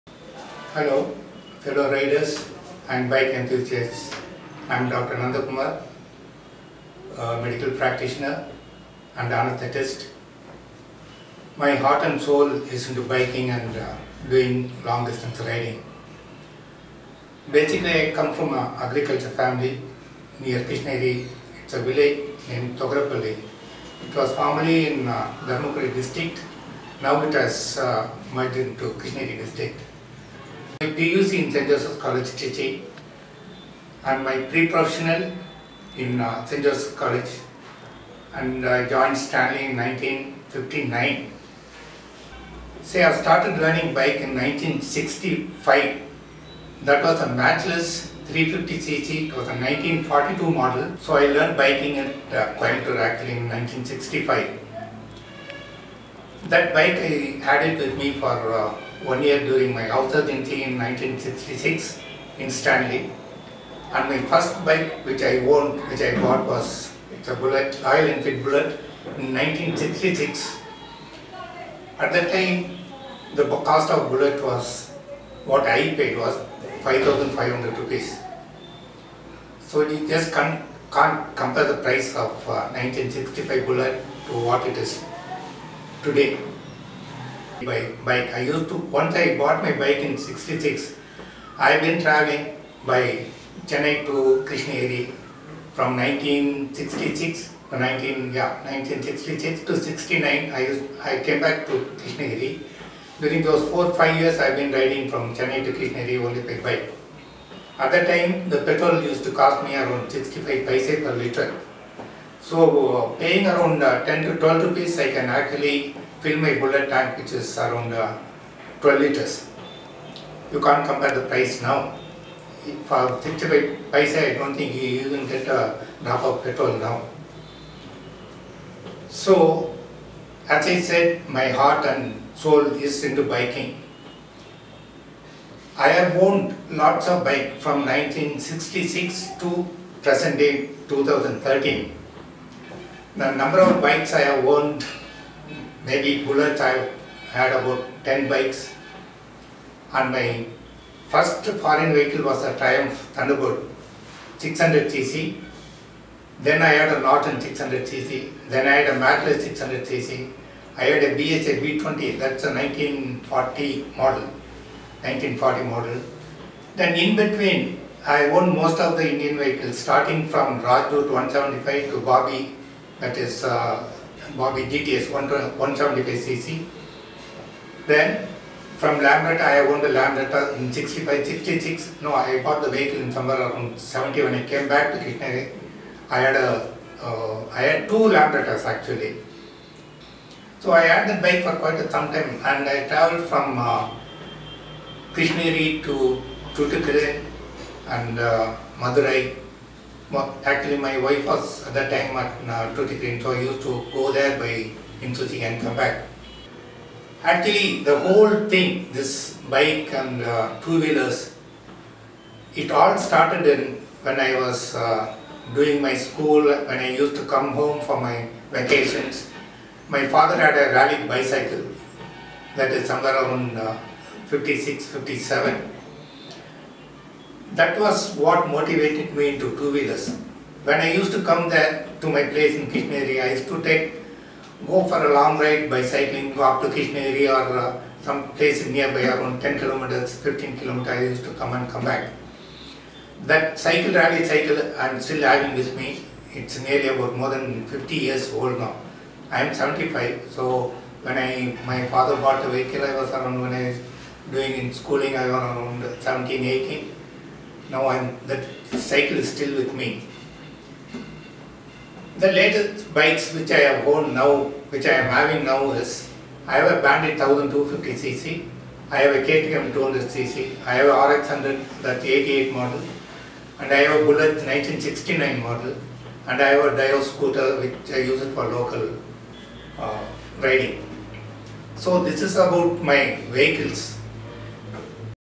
Wheeled Wanderers - Dr. Nandakumar's "Life in to Biking" - Part 1
Dr. Nandakumar  - A hard core biker lives in krishnagiri district, Tamilnadu. A self introduction about his remembrance of Biking experience.